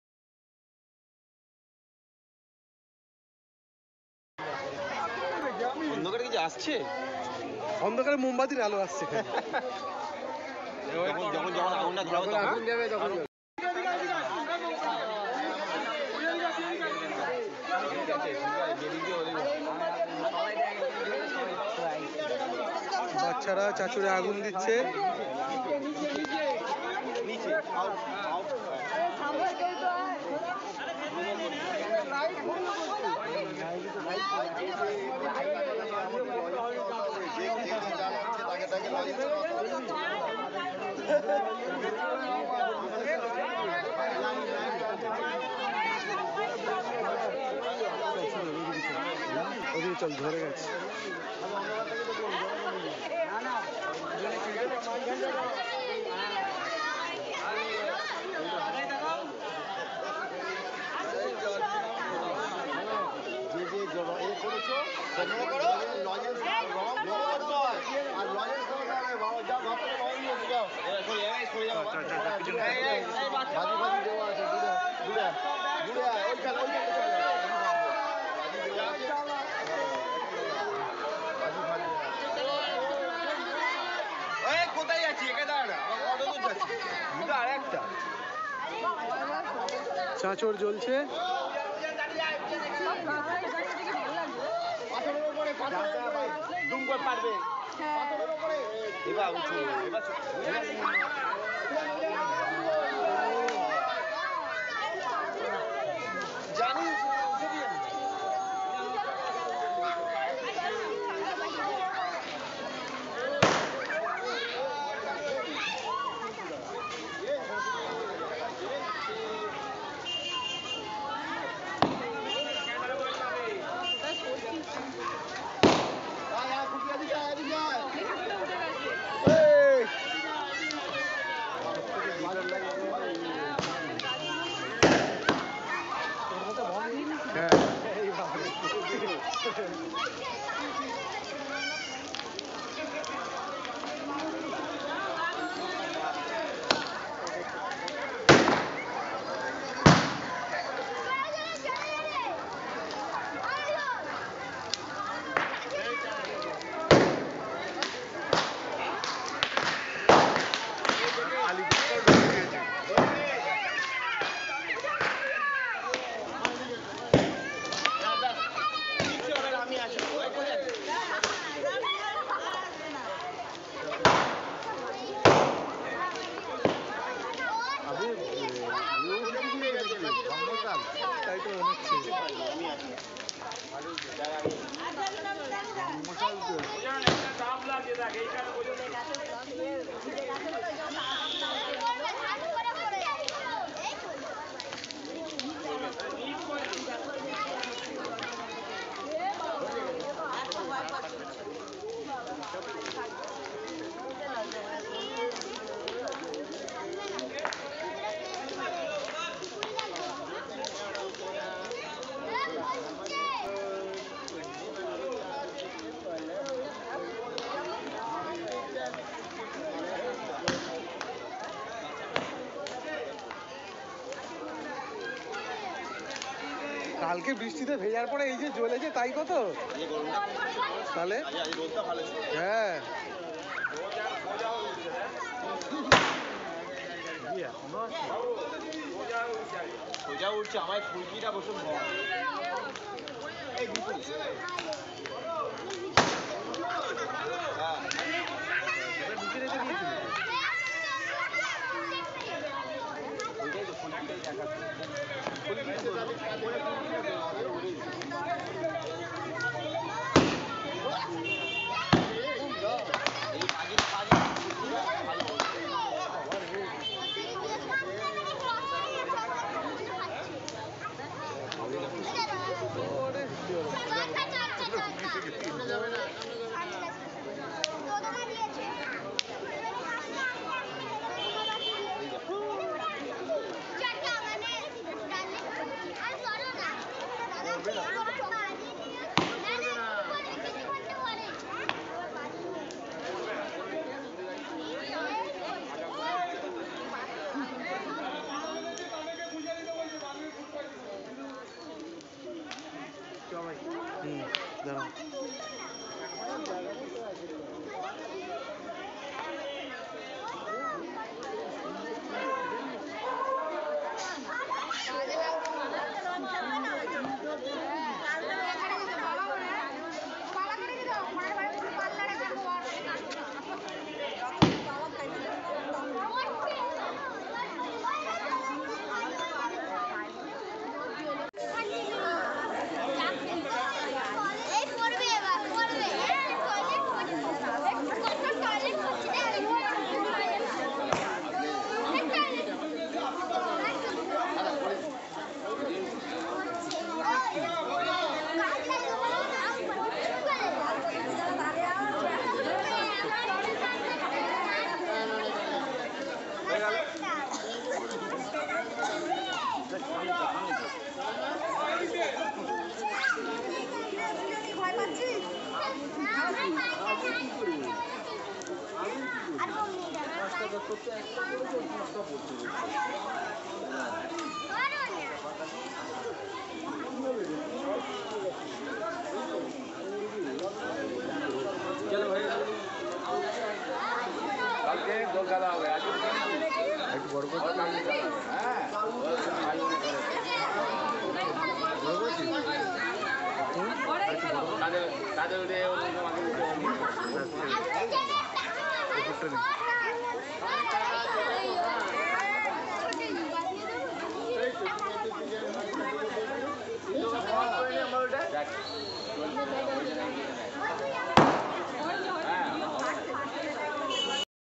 CHANCHOR#HOLIKA DAHAN#NERA PORA 2020
It's a fantastic ritual which is celebrating 1 day before holi. It has many name like CHANCHOR, NERA PORA, BURIR GHOR PORA, HOLIKA DAHAN etc. Science say, in this season many bacteria, virus , insects or germs are growing. By this ritual they are destroyed. But day by day this event is obsoleted 😔